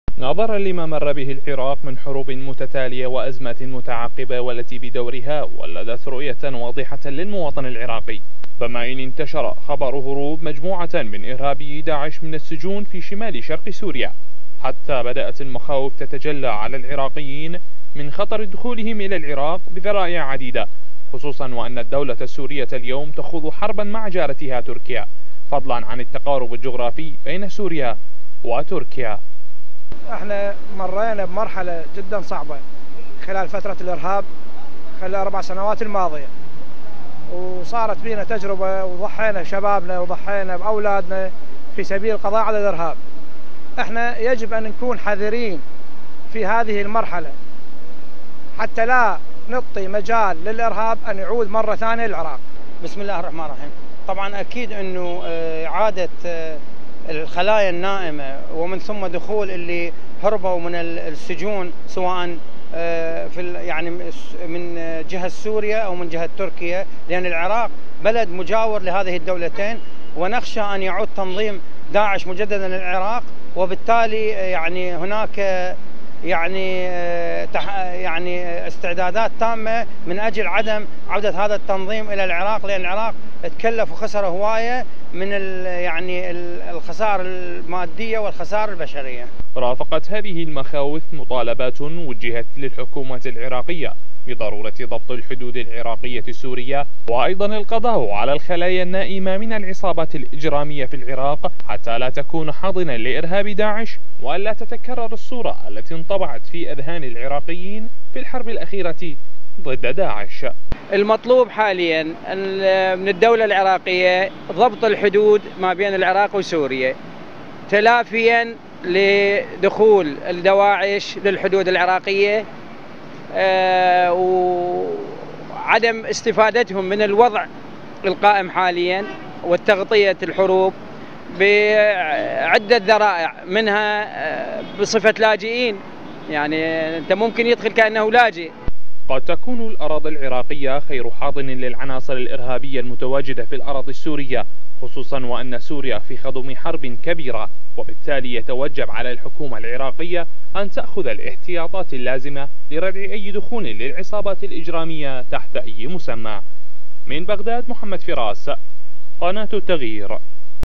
البغداديون يطالبون بتحصين الحدود بعد هروب عناصر داعش من مخيمات قسد | تقرير
هروب عناصر داعش من سجون شمال شرق سوريا والحرب التي تخوضها الحكومة السورية مع جارتها تركيا اثارت مخاوف كبيرة للشارع العراقي من خطورة عودة ارهاب داعش بعد فترة الاستقرار الذي يمر به العراق مطالبين الحكومة العراقية بتحصين الحدود العراقية وايضا القضاء على الخلايا الارهابية النائمة .